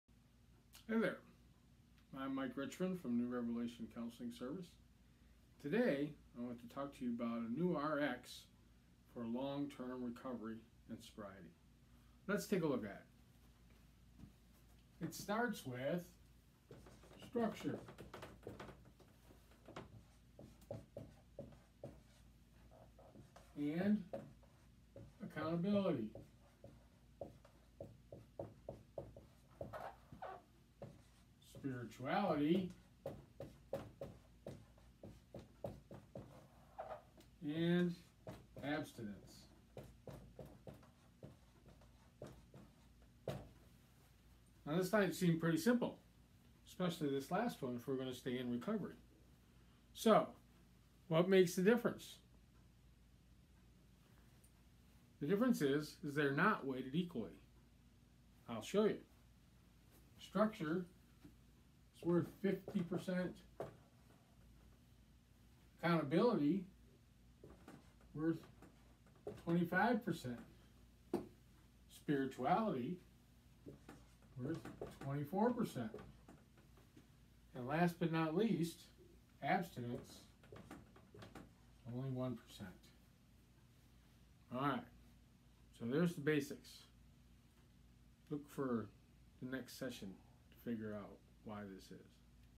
Basics of Recovery #1 Simple RX to Recovery
The Simple RX to recovery include 4 basic categories. Structure, Accountability, Spirituality, and Abstinence